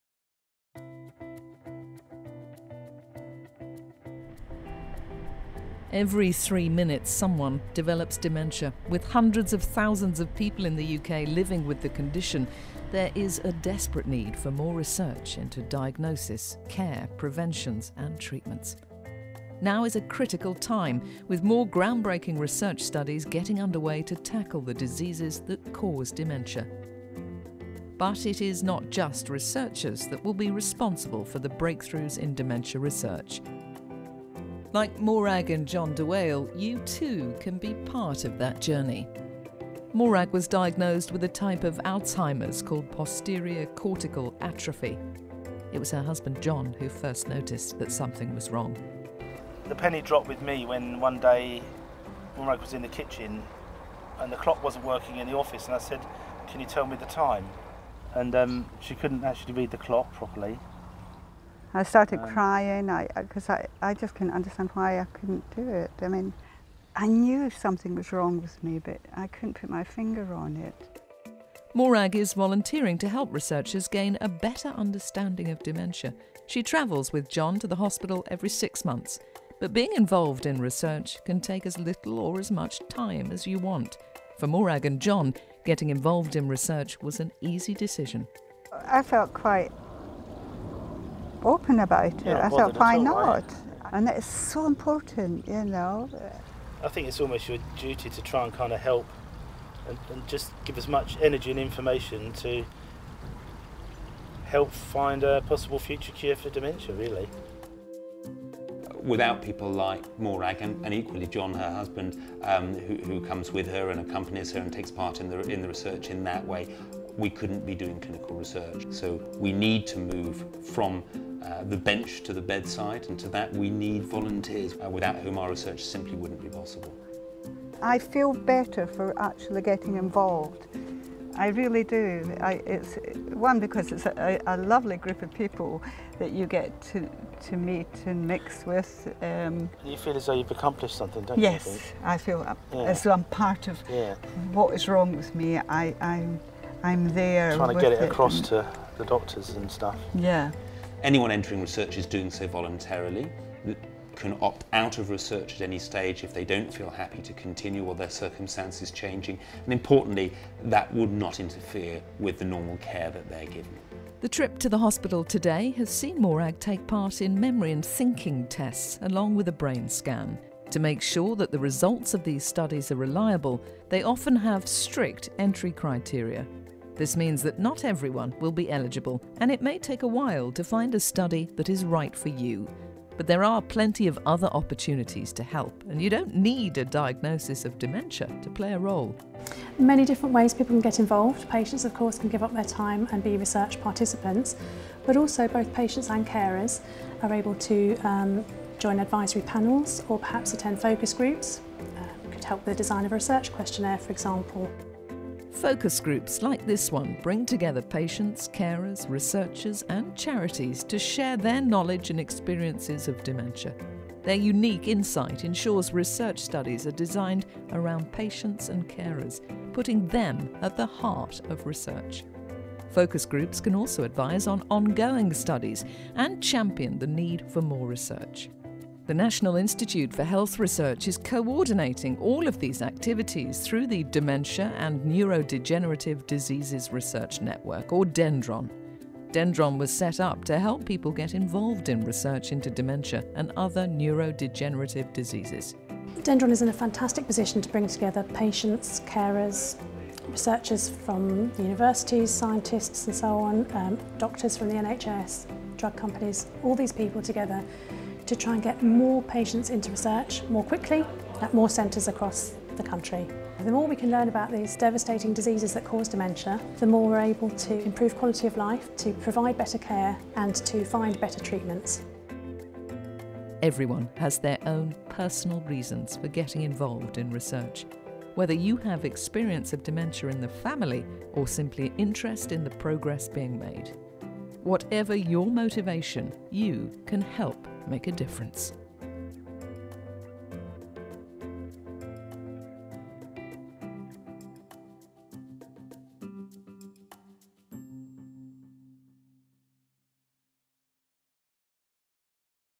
Taking part in dementia research
To mark World Alzheimer's Day on Saturday 21 September 2013, the National Institute for Health Research (NIHR) in partnership with Alzheimer's Research UK, has released a new short film which hopes to encourage more patients and carers to take part in ground-breaking research into dementia and other neurodegenerative diseases.